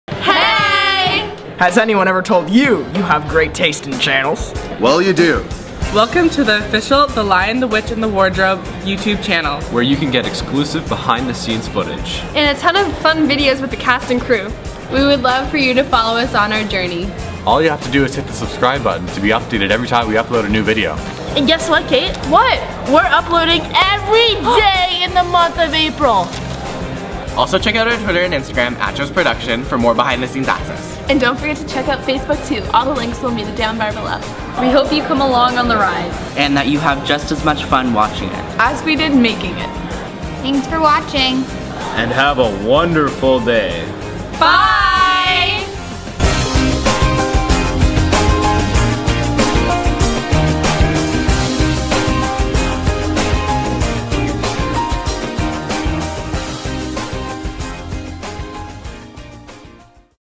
Welcome To The Channel
St. Joseph's Production of "The Lion, The Witch, and The Wardrobe" opens April 30th and May 1st. Tickets are $7 for students and $10 for adults. Hope to see you there!

Video Editor: Elle Mills